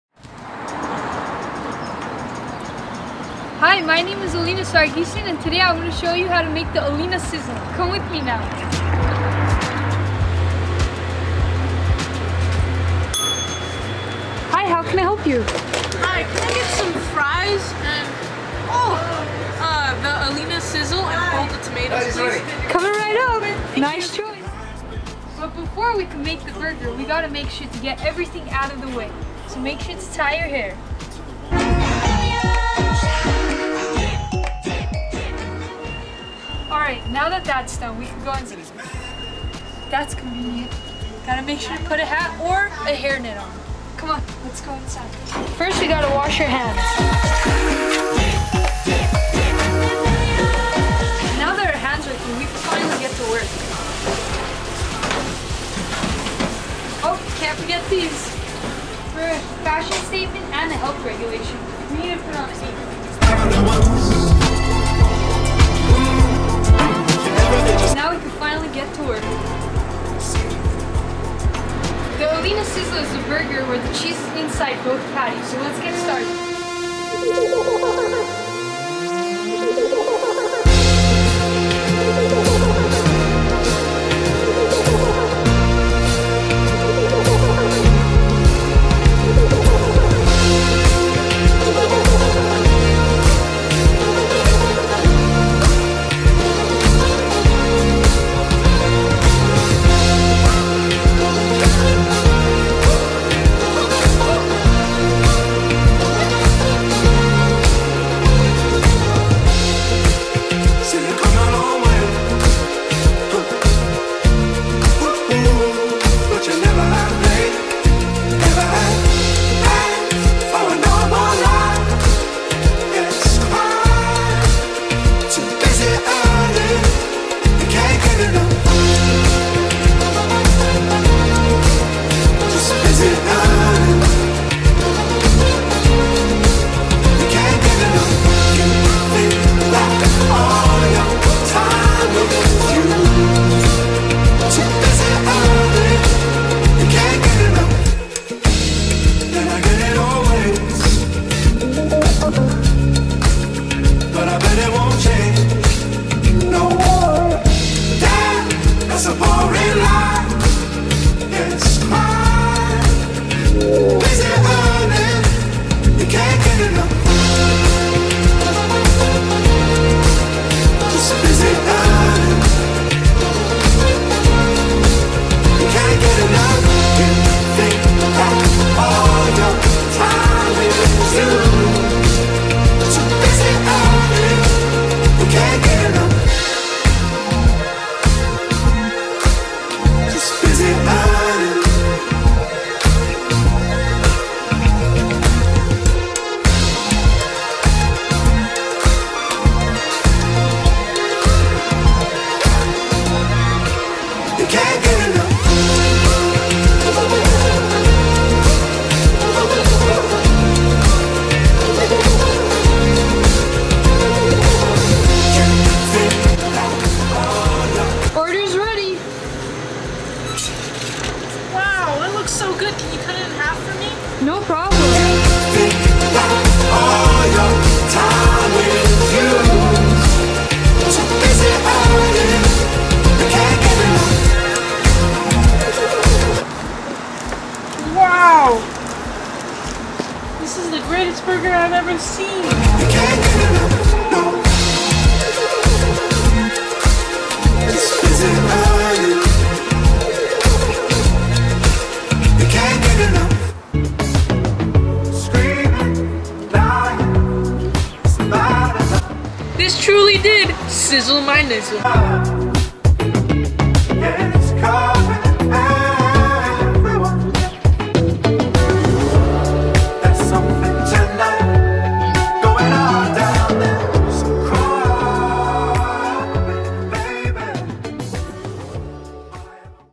Senior Project
This video is about my Senior Project at Clark Magnet High School.